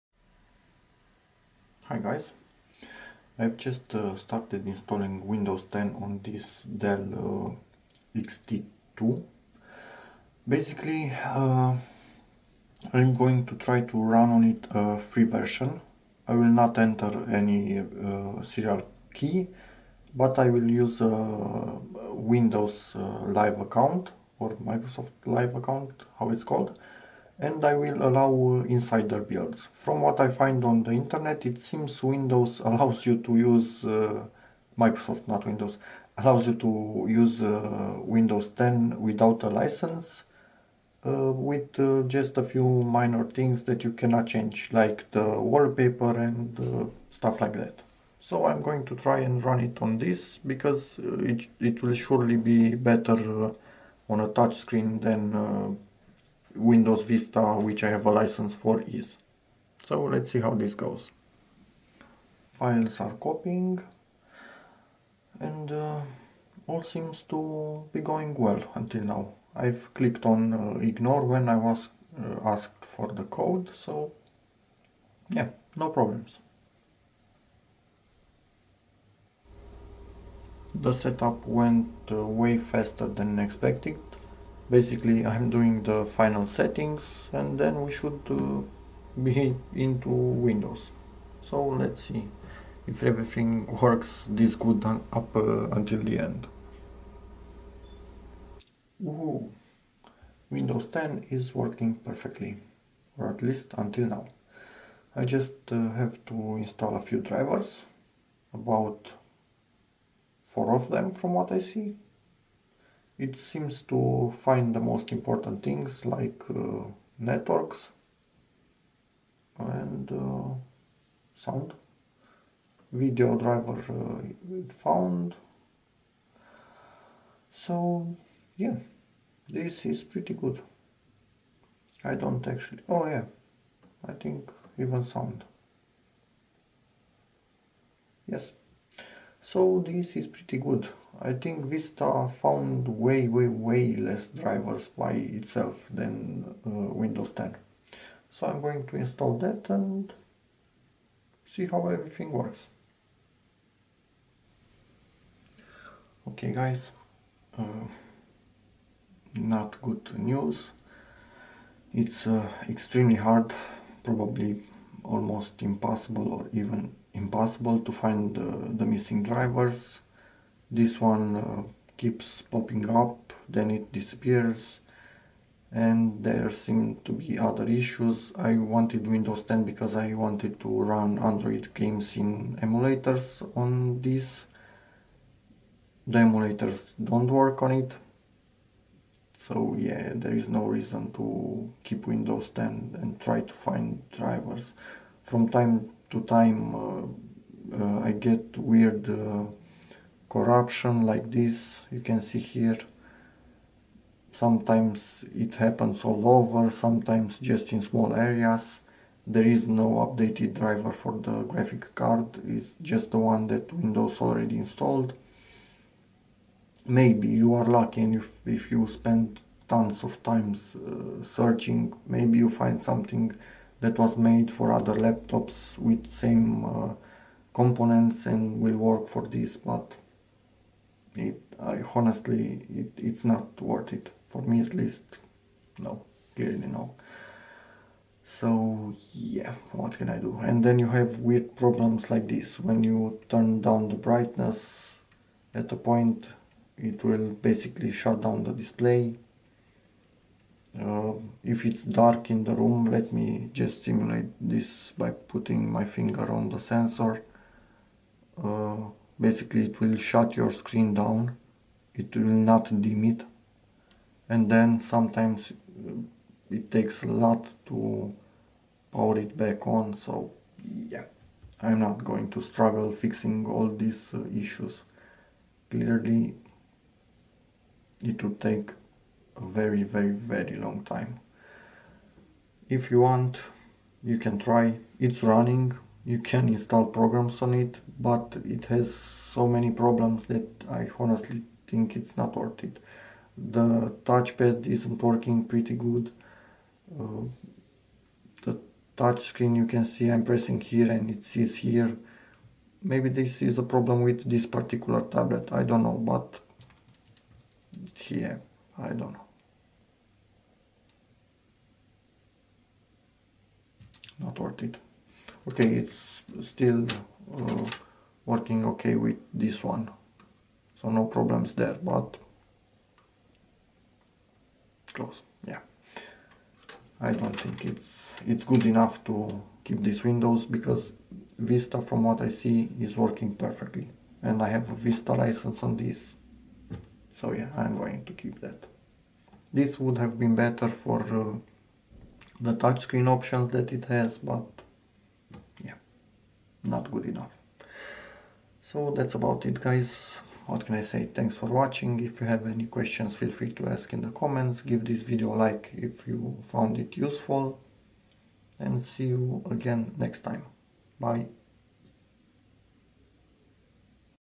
Windows 10 barely working on DELL XT2 convertible tablet
Maybe it can be made to work, but in this case it was not worth my time.